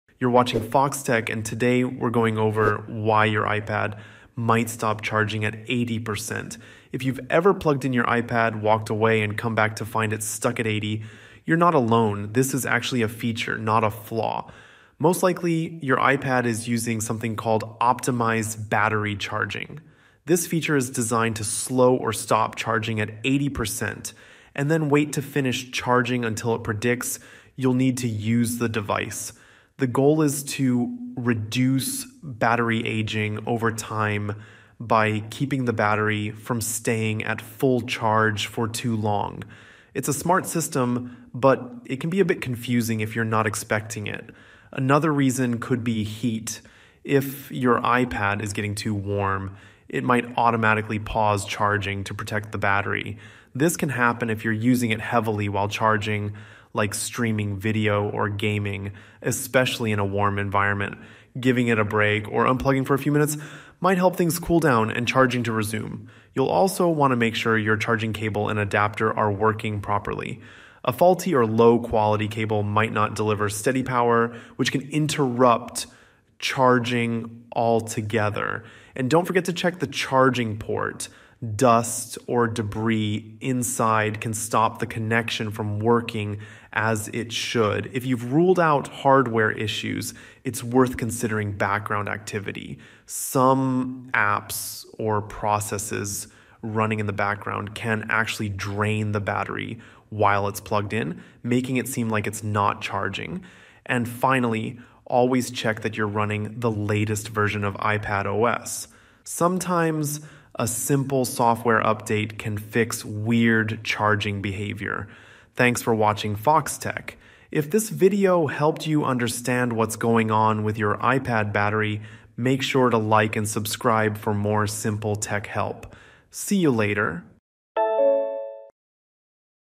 Why My iPad Stop Charging at 80%? (explained)
Wondering why your iPad stops charging at 80%? In this quick video, I’ll explain how Optimized Battery Charging works and how to manage this setting if you want a full charge every time!